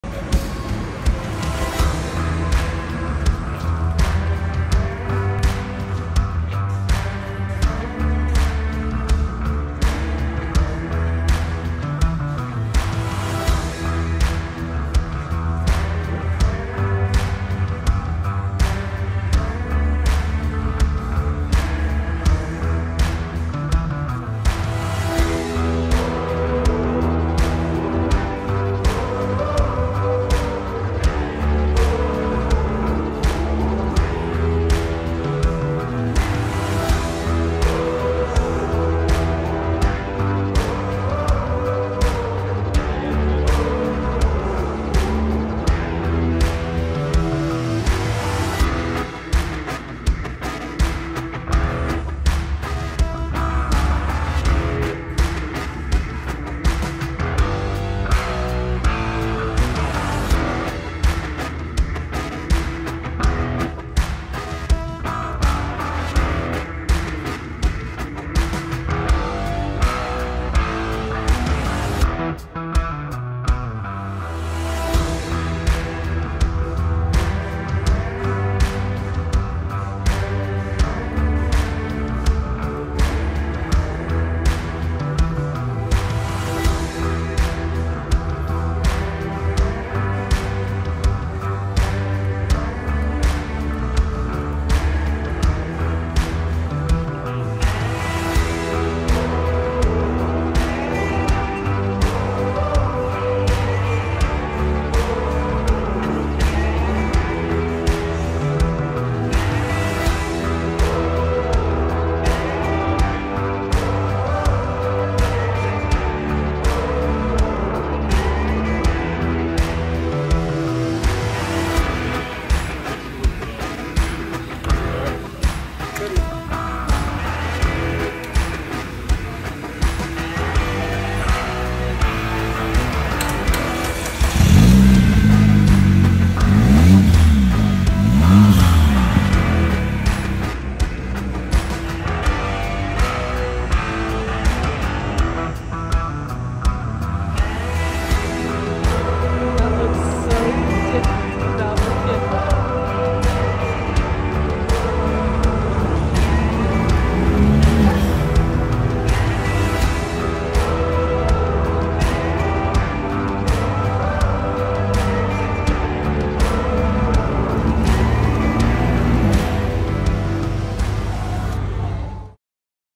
Visiting American Biker
Road-trip to American Biker/Indian Motorcycles of Charleston SC